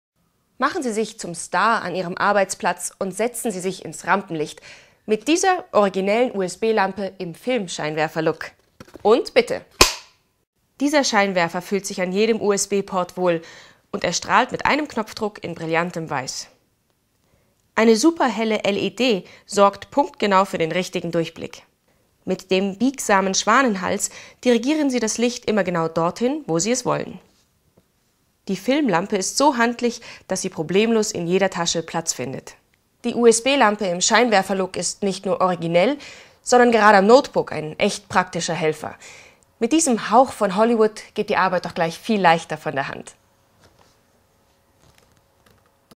Ersatz-Set mit 2 Scherfolien & 2 Rasierköpfen für USB-Rasierer
MachenSie Ihren Rasier wieder fit

Kein Problem, wenn der Rasierer mal unsanft gelandet ist oder die Scherfolie nach langer Benutzungszeit ausgetauscht werden muss.
Mit diesem Ersatzset machen Sie Ihren Rasierer wieder für lange Zeit fit.